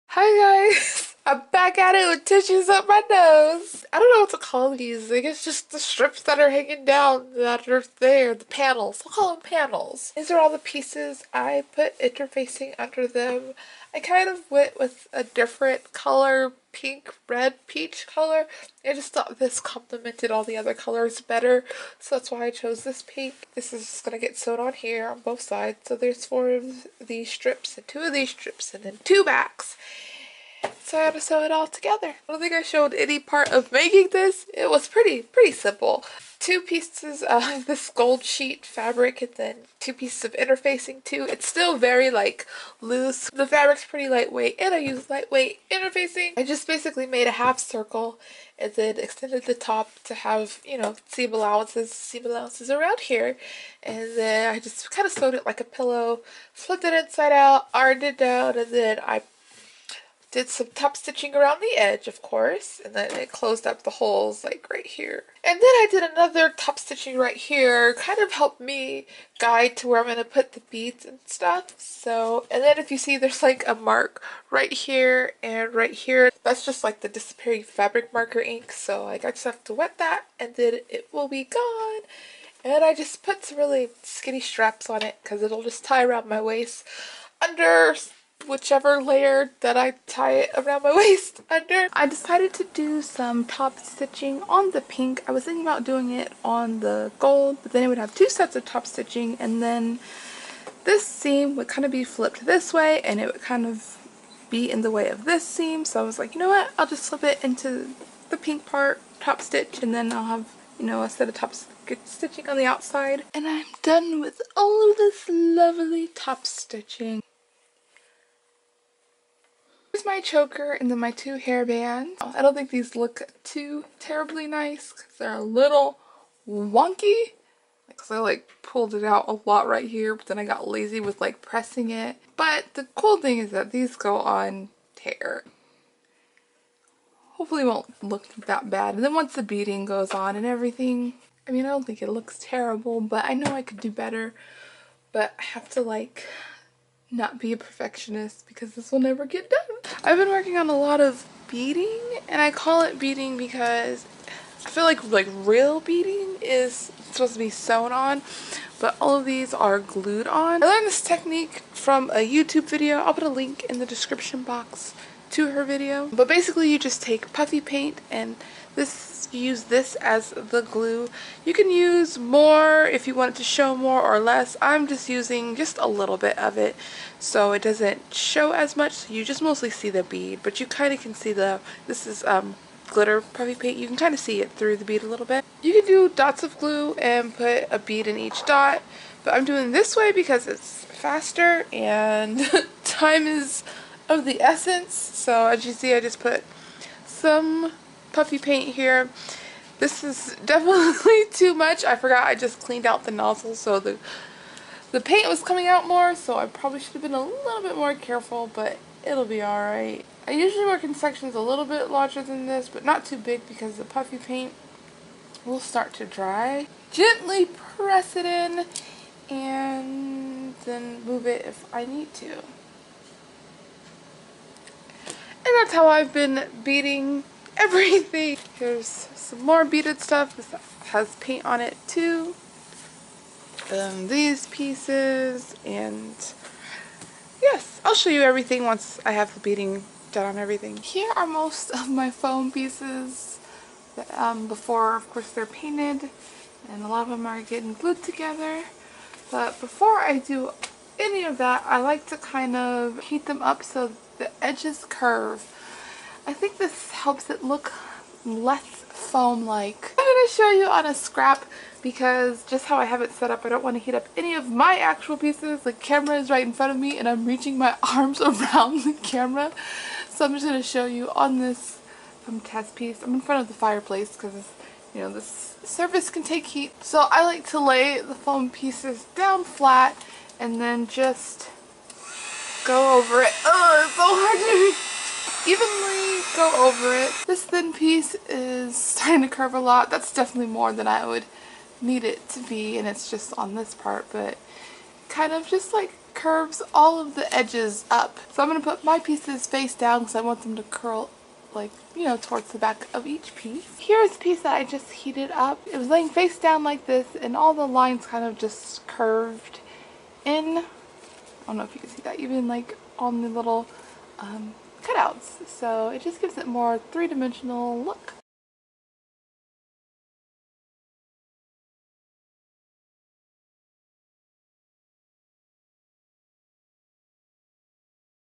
(4) Creating Princess Apricot: Panels, "Beading", Foam Shaping, & Belt
Allergies snatched me up again so this video has plenty of mouth breathing. This is the last of the footage I took while creating this cosplay. The convention was in a few days and I had to rush to finish which is what I did NOT want to do. Hopefully I truly learned my lesson especially  since I have it in video format, lol.